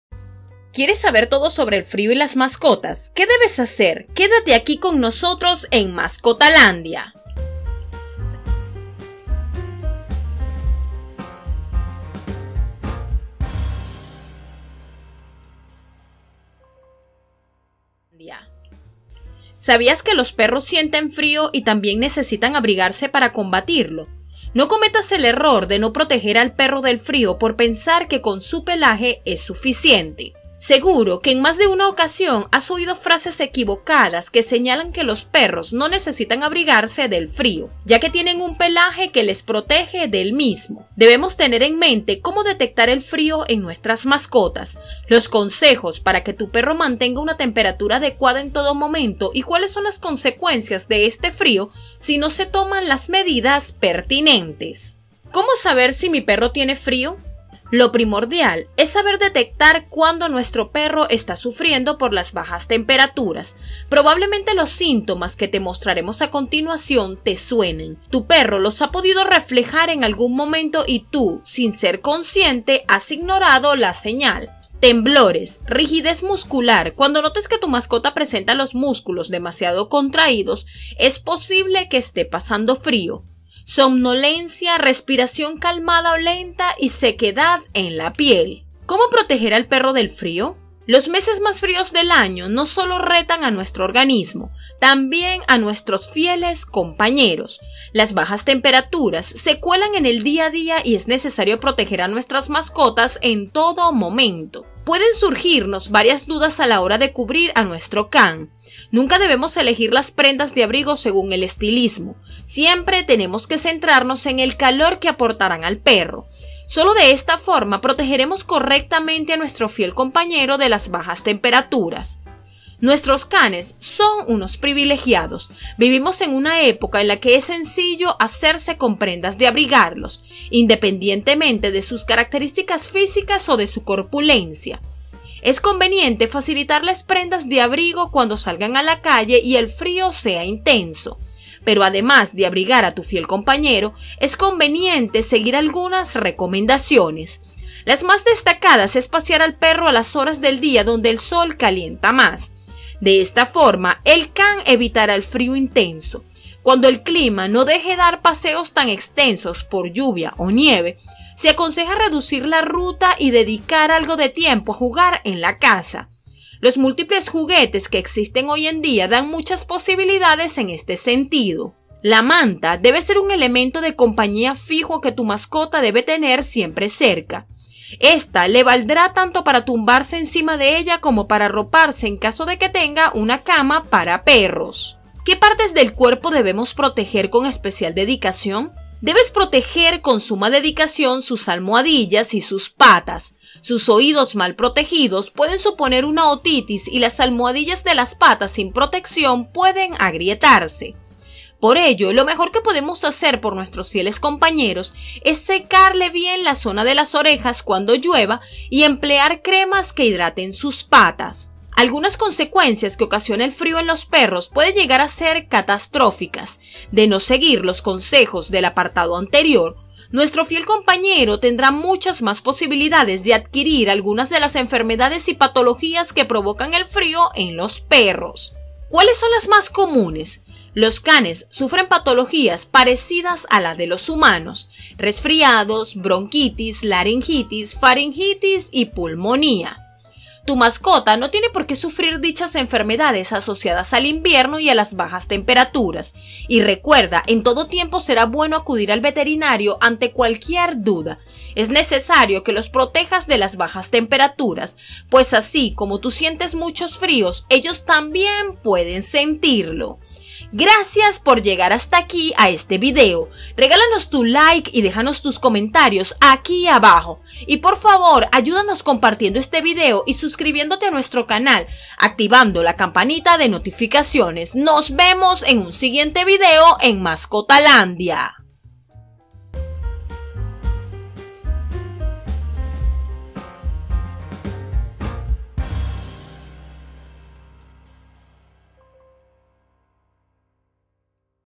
El Frio y las MASCOTAS ¿Que Debes Hacer?
Muchos confían en el pelaje de la mascota pero sabes, ¿que debes hacer ante el frio y como protegerlos? quédate en el siguiente video que te lo voy a explicar.